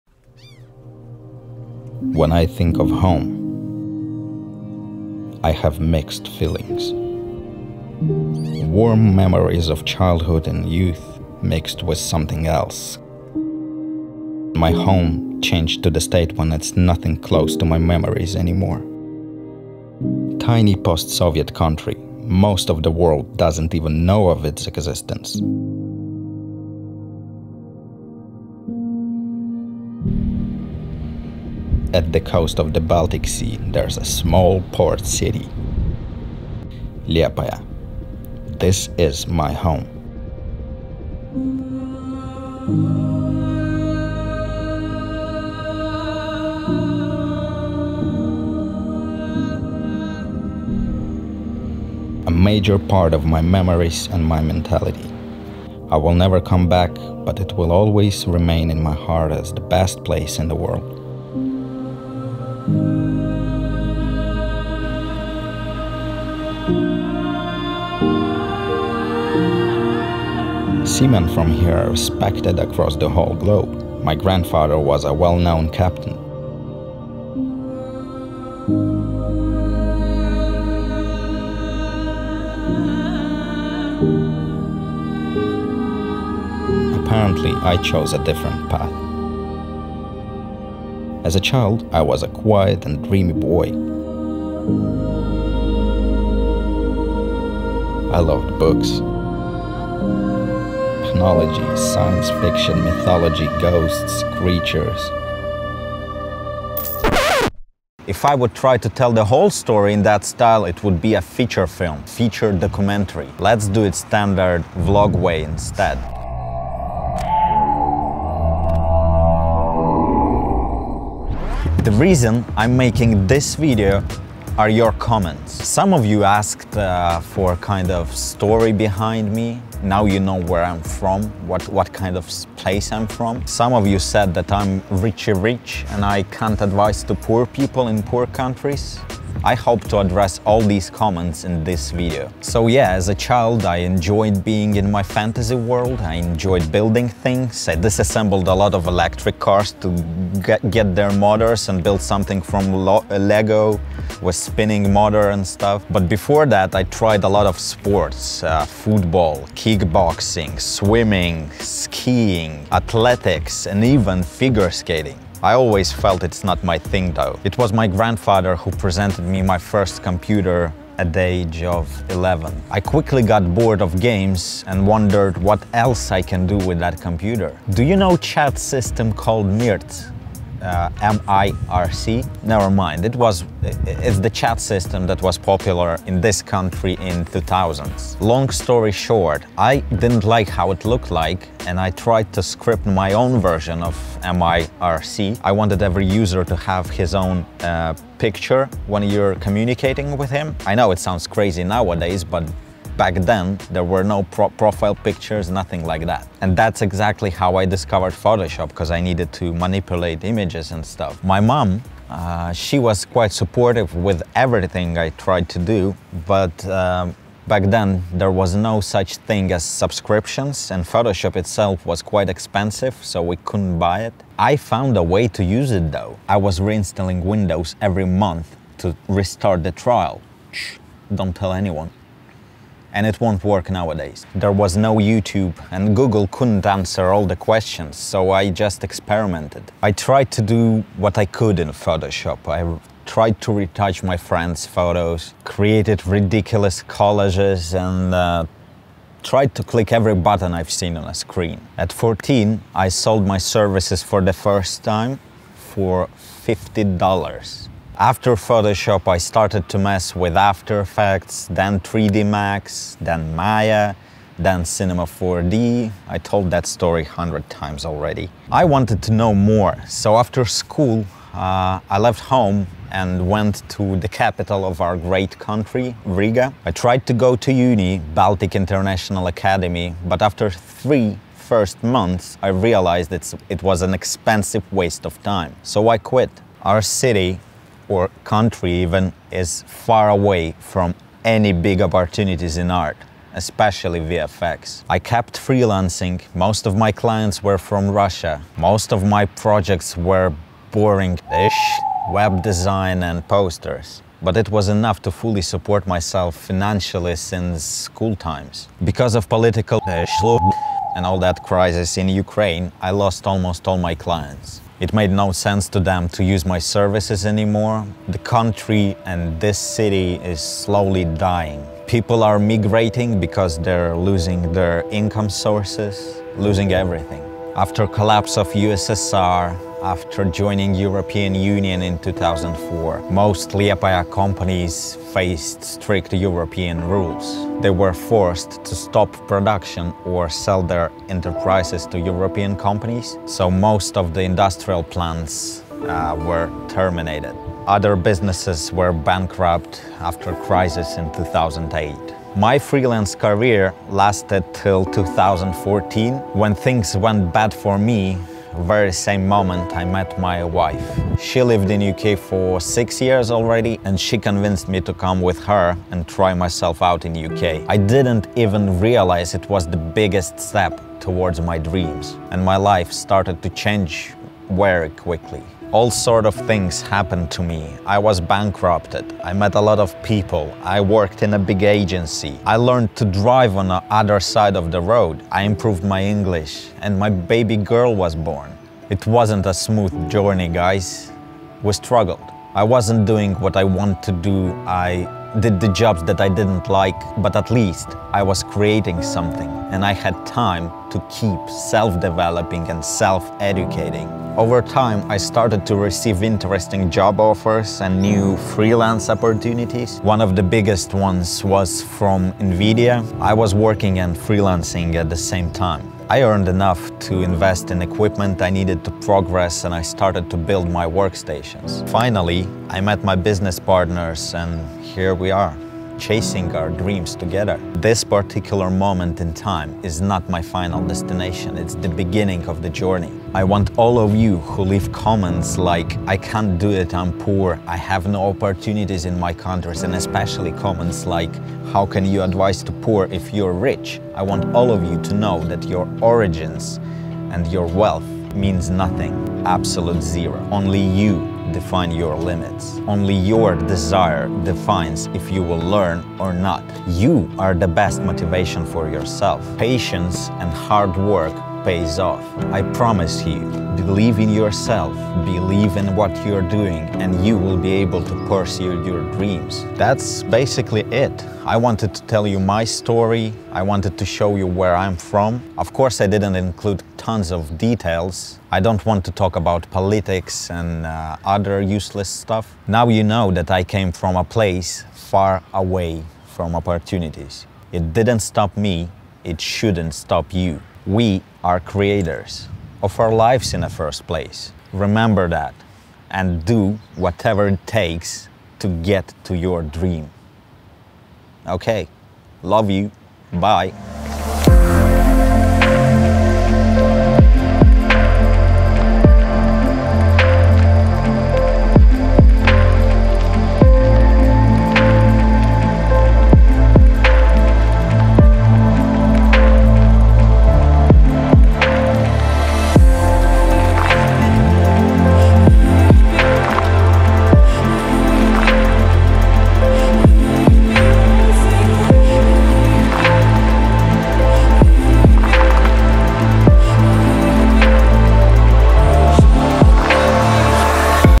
FAR AWAY from OPPORTUNITIES. Where I am from?
In this video, I want to take you on the trip to a very special place. Special for me of course. My home. I would like to show you that your origins and your wealth mean nothing. If you want to learn, if you want to grow - you will.

My country is far away from any CG&VFX opportunities. I found a way. You can find your way too. Let me know what you think about it in the comments, I would really appreciate it as this little video means a lot.

and/or by David Cutter music.